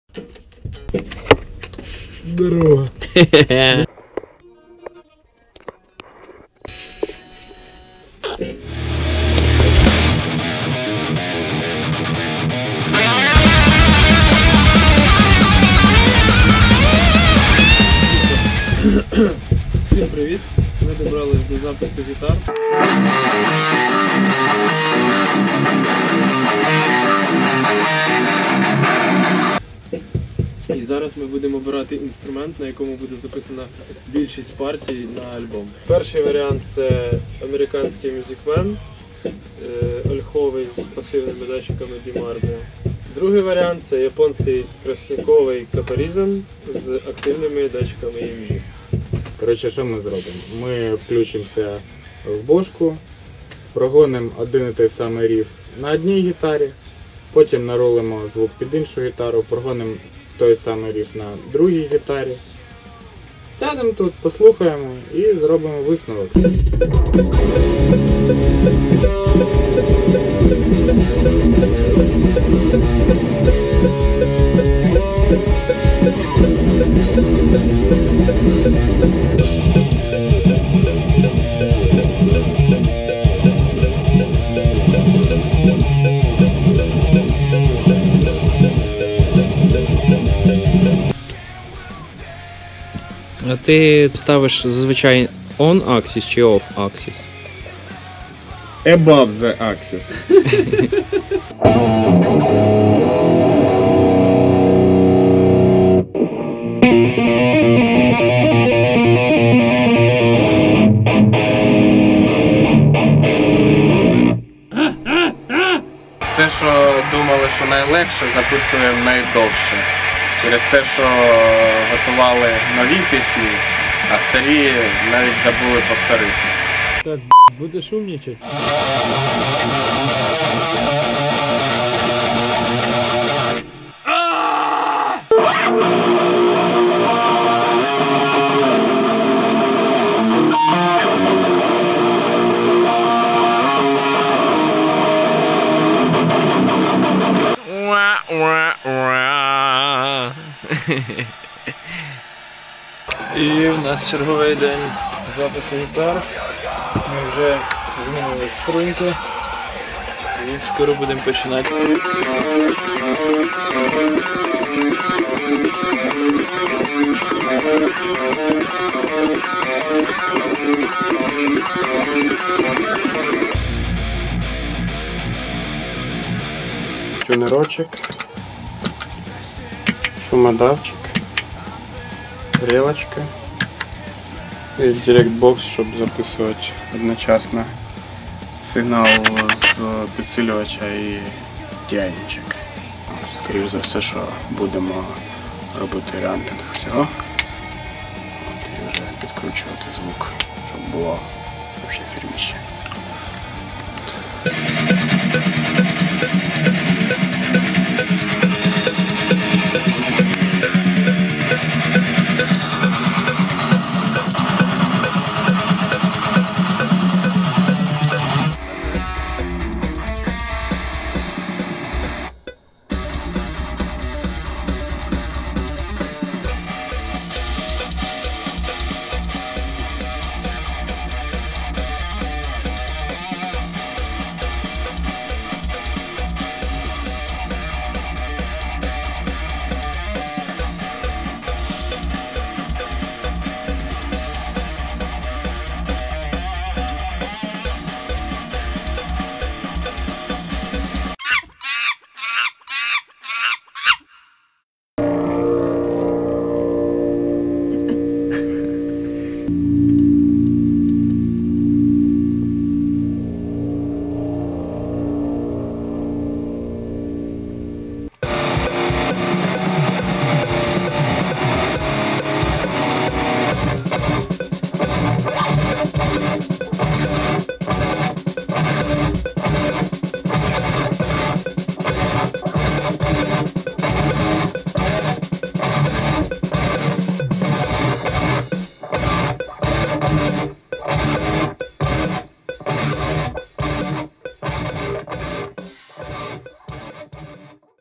Blackmail vlog Part3 | Guitars
Third part of series of short segments regarding the production side of Blackmail's first full-length album. In this part we show guitars recording process.